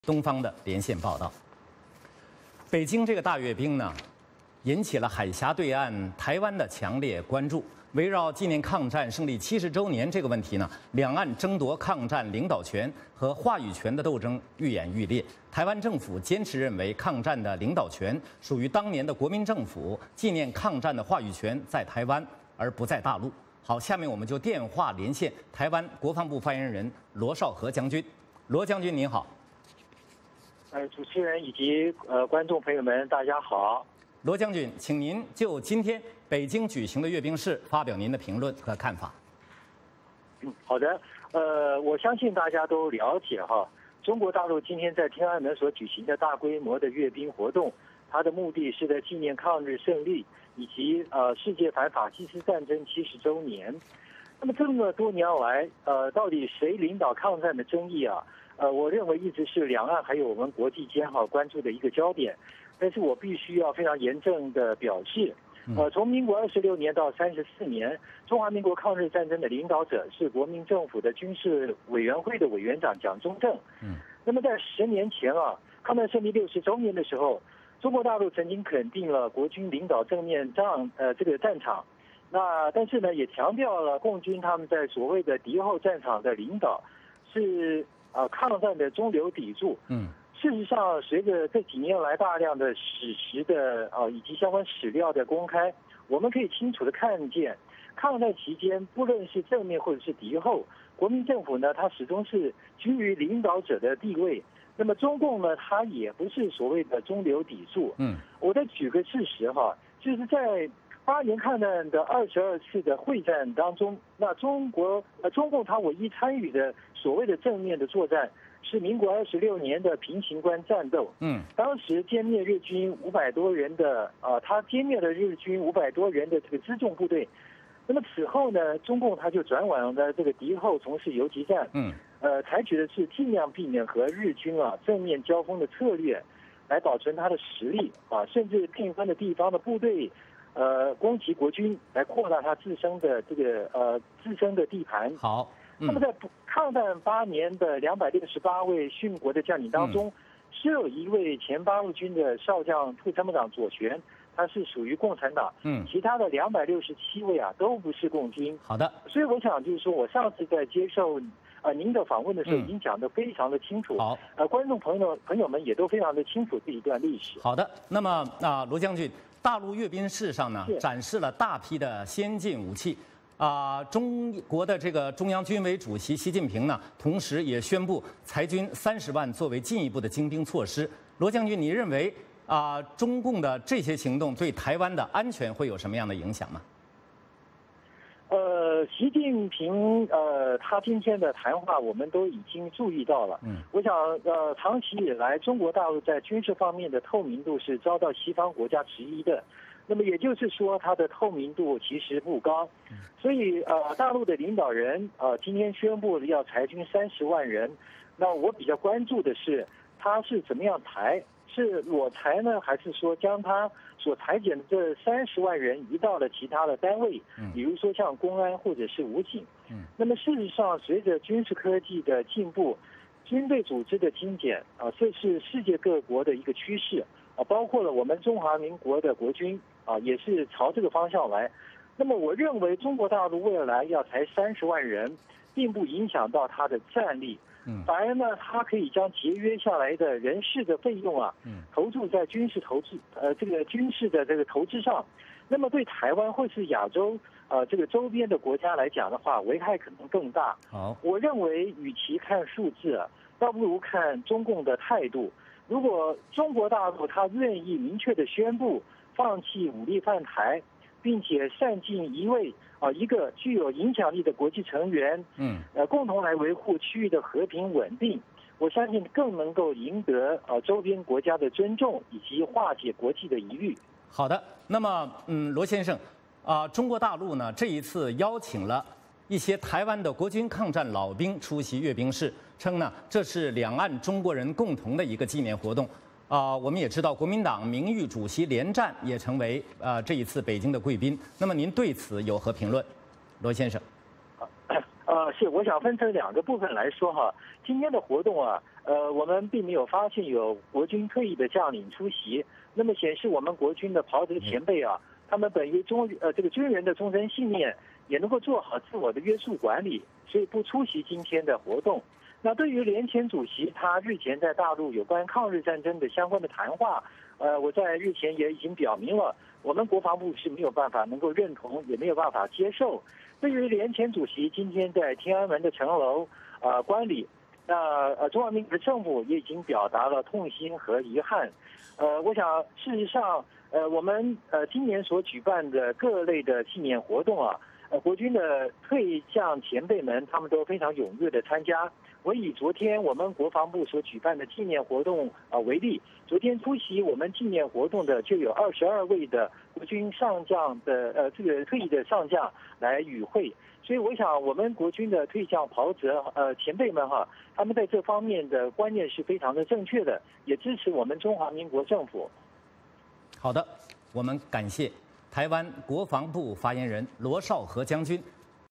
VOA连线：台湾国防部：纪念抗战的话语权在台湾
北京的大阅兵同样引起海峡对岸的台湾的强烈关注，围绕纪念抗战胜利70周年这个问题，两岸争夺抗战领导权和话语权的斗争愈演愈烈，台湾政府坚持认为，抗战的领导权属于当年的国民政府，纪念抗战的话语权在台湾，不在大陆。